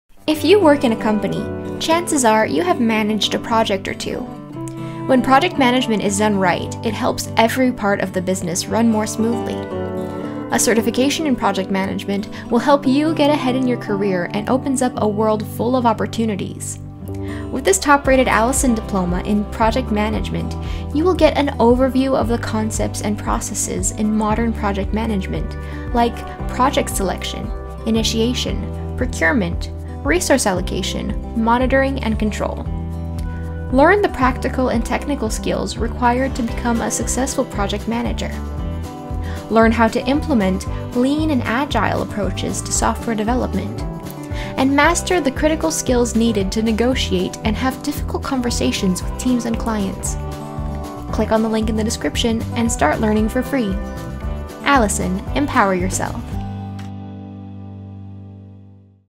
Diploma in Project Management in Practice - Free Online Course with Certificate
In this video, you will learn about Alison’s Diploma- Project Management in Practice course.

This online technical project management course will teach you about practical project management. It covers the fundamental principles and practices in project management including basic concepts such as project selection, initiation and procurement. You will study leadership, planning, scheduling, risks, and costs and budgets. You will also learn about resource allocation, monitoring, and control needed to manage complex technical projects.